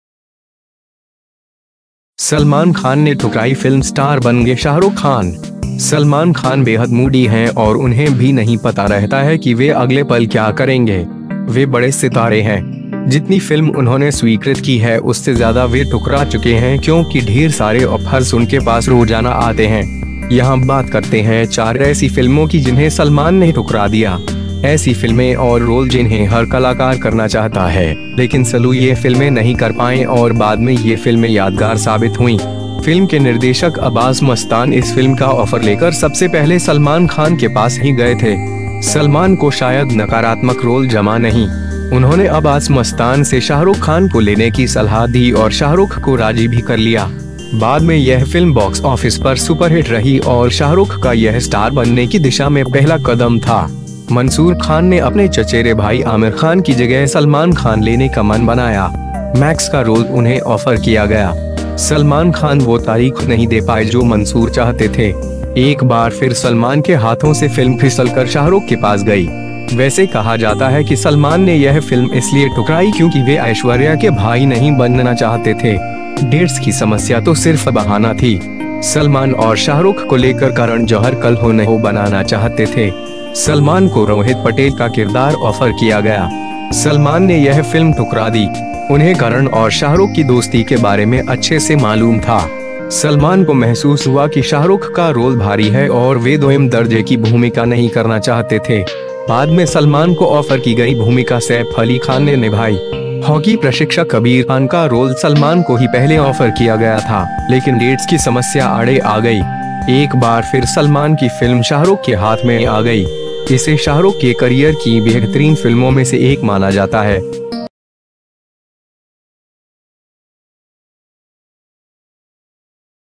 सलमान खान ने ठुकराई फिल्म स्टार बन गये शाहरुख खान !
सलमान खान बेहद मूडी हैं और उन्हें भी नहीं पता रहता है कि वे अगले पल क्या करेंगे. वे बड़े सितारे हैं.
यहां बात करते हैं 4 ऐसी फिल्मों की जिन्हें सलमान ने ठुकरा दिया. ऐसी फिल्में और रोल जिन्हें हर कलाकार करना चाहता है, लेकिन सल्लू ये फिल्में नहीं कर पाएं और बाद में ये फिल्में यादगार साबित हुईं.

सलमान को शायद नकारात्मक रोल जमा नहीं. उन्होंने अब्बास-मस्तान से शाहरुख खान को लेने की सलाह दी और शाहरुख को राजी भी कर लिया. बाद में यह फिल्म बॉक्स ऑफिस पर सुपरहिट रही और शाहरुख का यह स्टार बनने की दिशा में पहला कदम था.

मंसूर खान ने अपने चचेरे भाई आमिर खान की जगह सलमान खान लेने का मन बनाया. सलमान खान वो तारीख नहीं दे पाए जो मंसूर चाहते थे. एक बार फिर सलमान के हाथों से फिल्म फिसल कर शाहरुख के पास गई. वैसे कहा जाता है कि सलमान ने यह फिल्म इसलिए ठुकराई क्योंकि वे ऐश्वर्या के भाई नहीं बनना चाहते थे. डेट्स की समस्या तो सिर्फ बहाना थी.

सलमान और शाहरुख को लेकर करण जौहर ‘कल हो न हो’ बनाना चाहते थे. सलमान ने यह फिल्म ठुकरा दी. उन्हें करण और शाहरुख की दोस्ती के बारे में अच्छे से मालूम था. सलमान को महसूस हुआ कि शाहरुख का रोल भारी है और वे दोयम दर्जे की भूमिका नहीं करना चाहते थे.